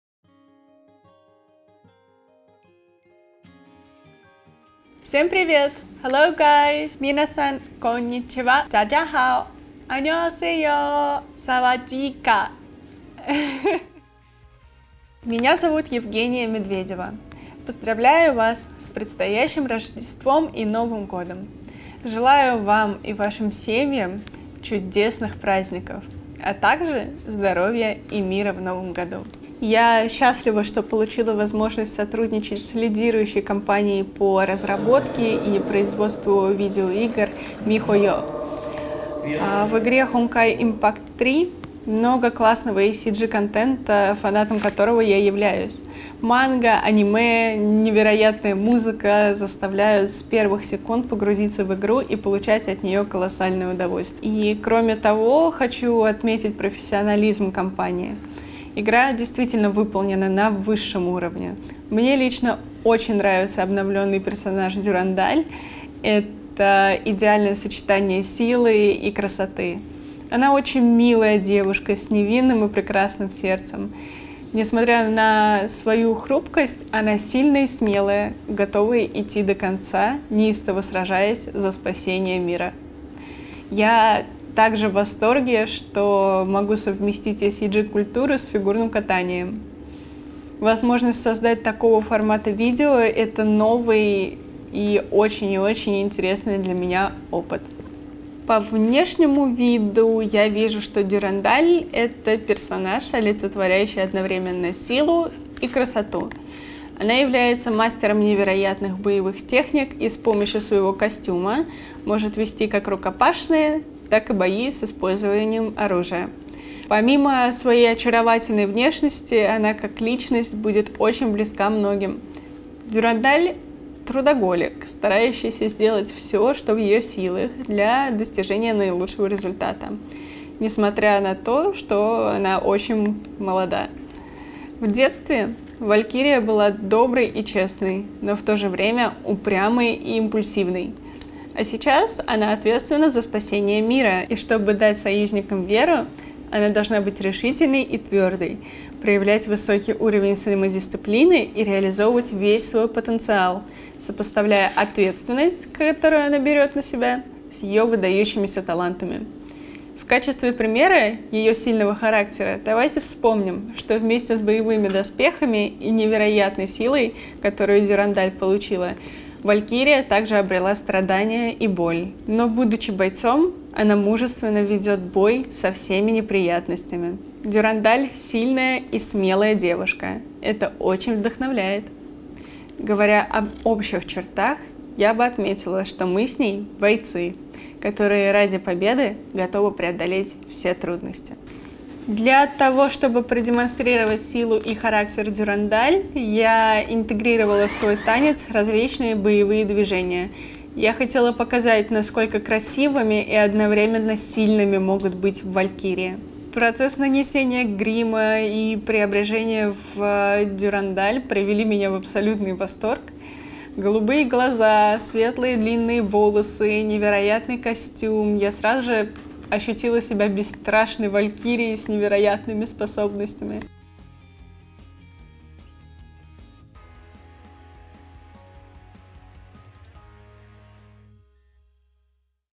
Honkai Impact 3rd x Evgenia Medvedeva: Interview Video - Honkai Impact 3rd
Merry Christmas, Captains!
We invited world figure skating champion Evgenia Medvedeva to talk about how we created the figure skating concept short film for [Dea Anchora] together!